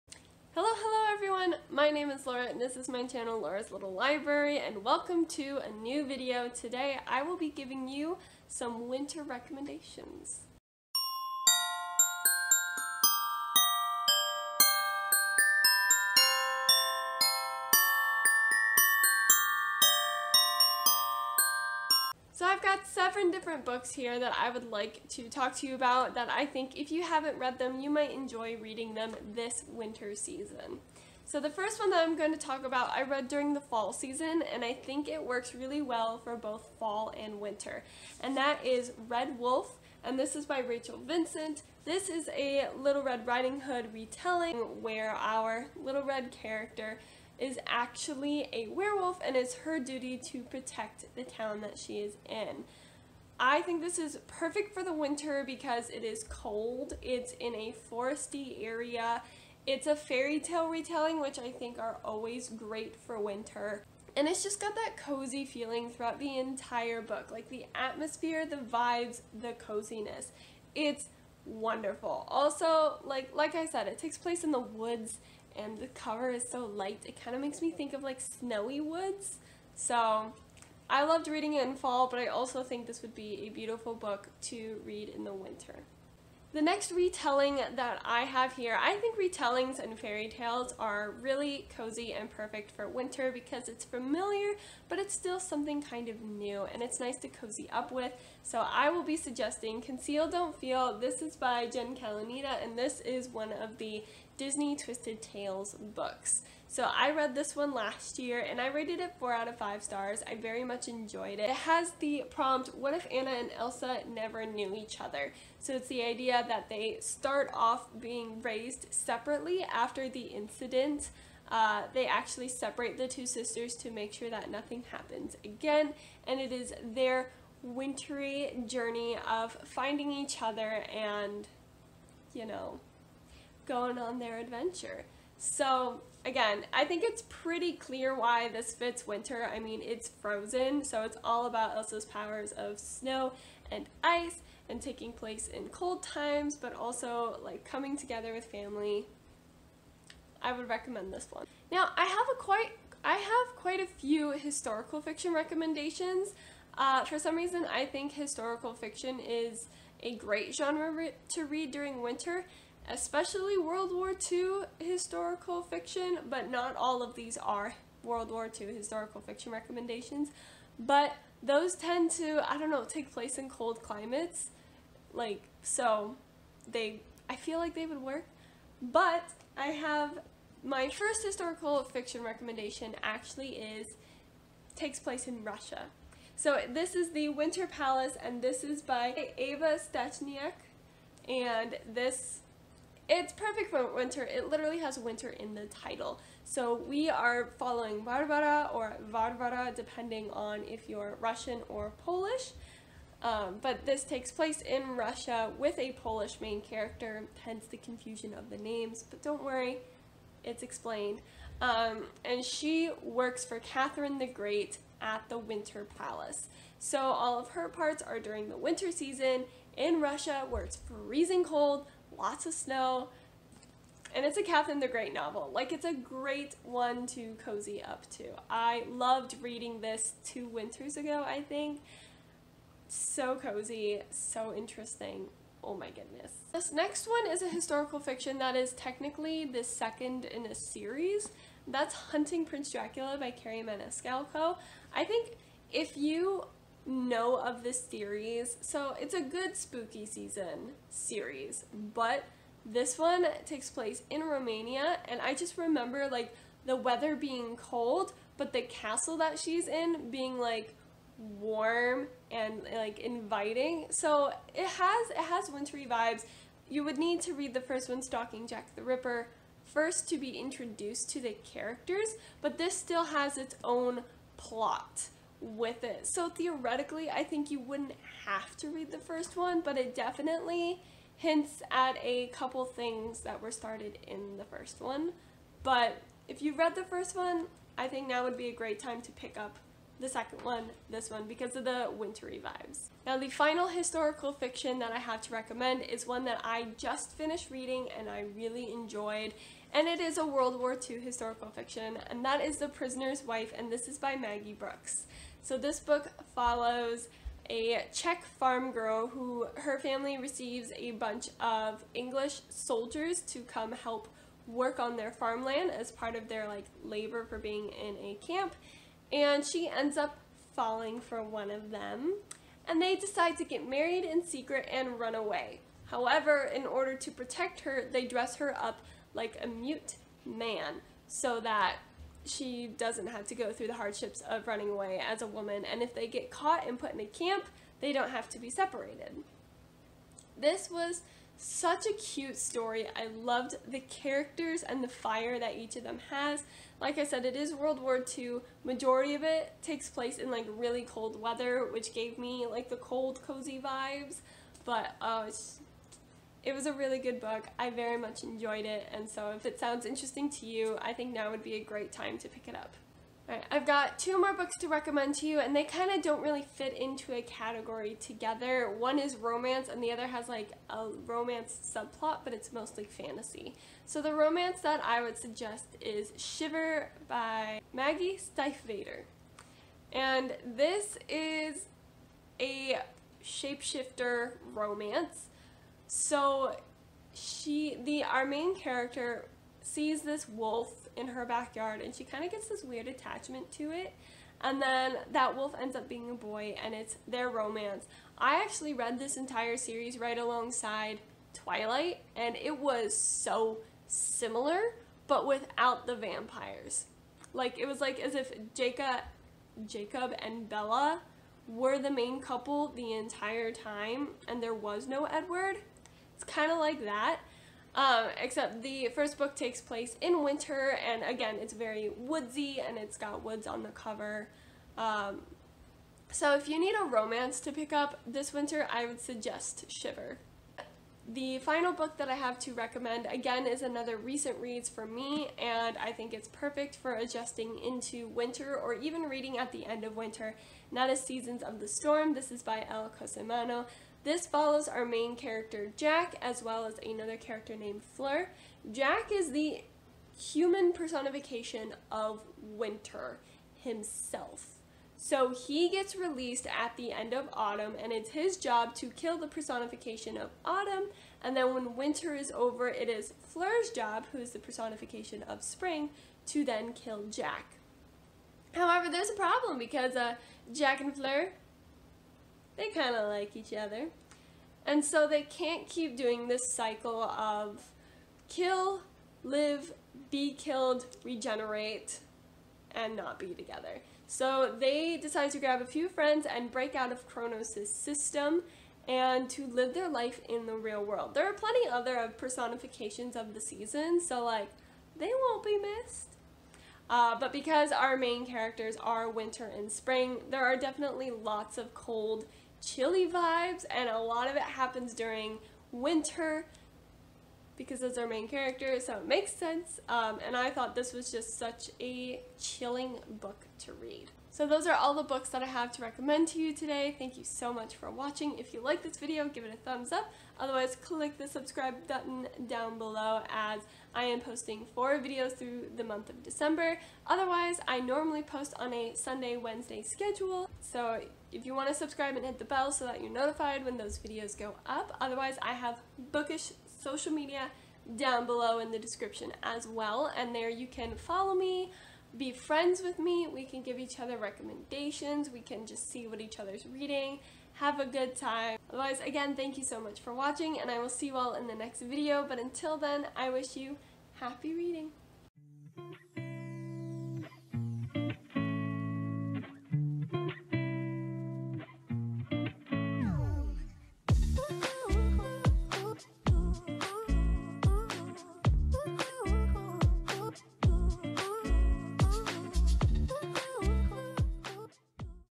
Winter Recommendations | Books to Read in Winter
Here are some books that would be good to read in the cold weather season!

0:00 Intro
0:35 Retellings and Fairytales
2:30 Historical Fiction
6:03 Other (romance and fantasy)
8:35 Ending

Storygraph: lauraslillibrary
Goodreads: Laura's Lil Library